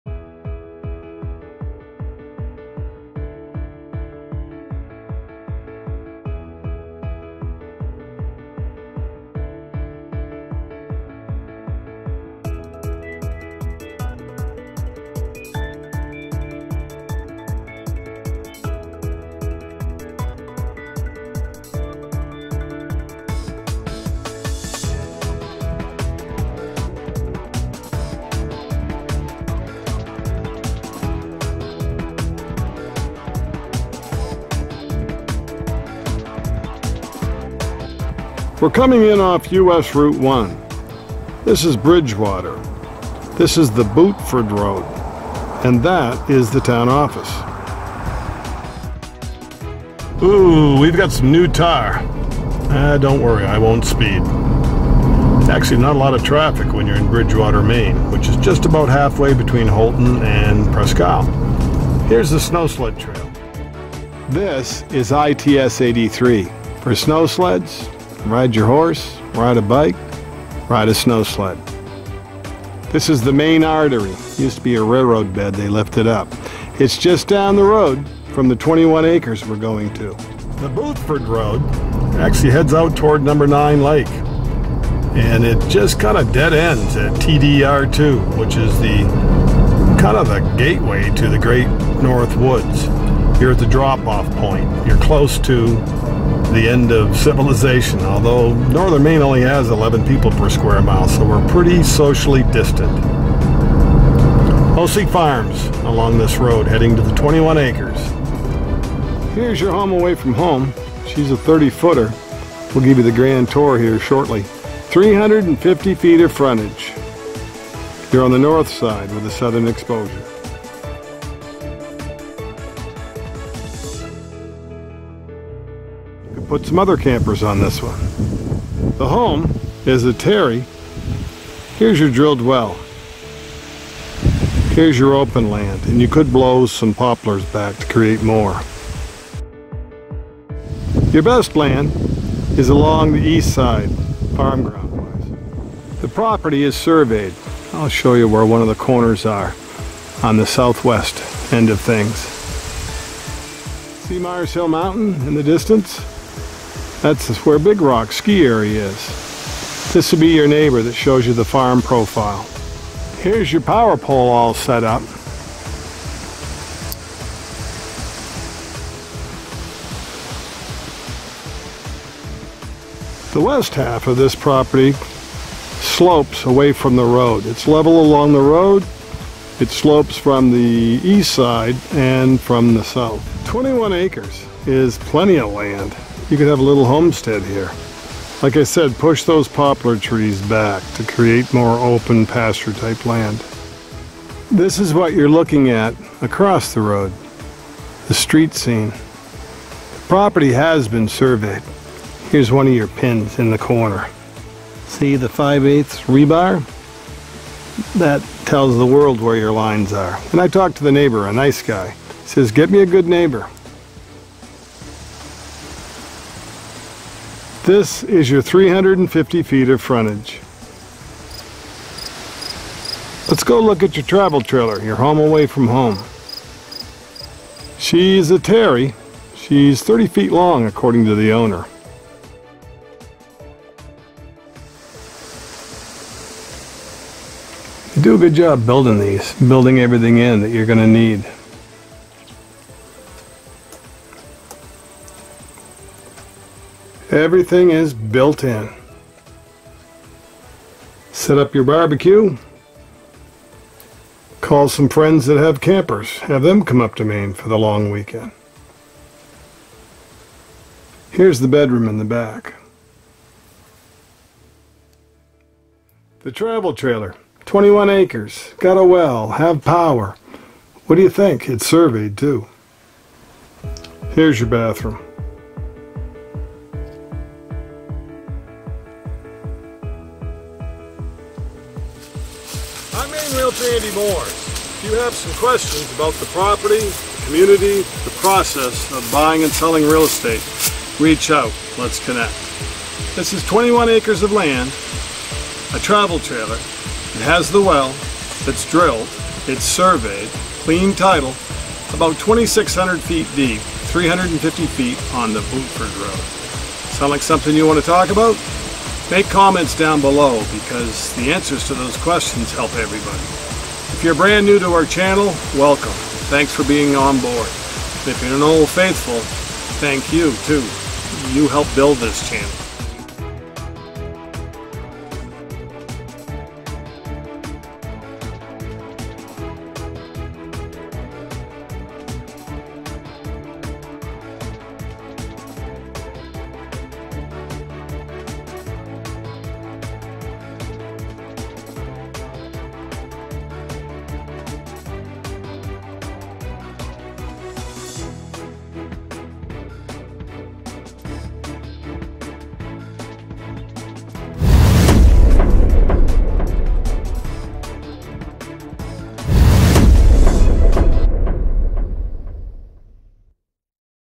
Land In Maine For Sale | Real Estate Video | 21 Acres, Trailer MOOERS REALTY #8970
Land In Maine For Sale, Real Estate Videos Shows You 21 Acres, Trailer. This Property Listings New To The Maine Real Estate Market Is On The North Side Of The Bootfoot Road, Bridgewater Maine. Half Mile Deep, 350' On Bootfoot Road Bridgewater Maine 21 Acres Of Land For Sale! Drilled Well On The Maine Land Is 300' Deep, Power Pole In Place. Real Estate Sale Includes 30' Long Terry Travel Trailer! Near #9 Lake, Mars Hill Mountain's Big Rock Ski Area. Handy To All The Maine Recreation Trail System Hook Ups For Snowmobiling, ATV Four Wheeling. Hunt, Fish, Build Home Here And Homestead! Brook Crosses This Maine Land! $27,500! Learn More On Land For Sale In Maine and Visit Link For Maine Real Estate Maine Is The Way Life Should Be! MOOERS REALTY 69 North ST Houlton ME 04730 USA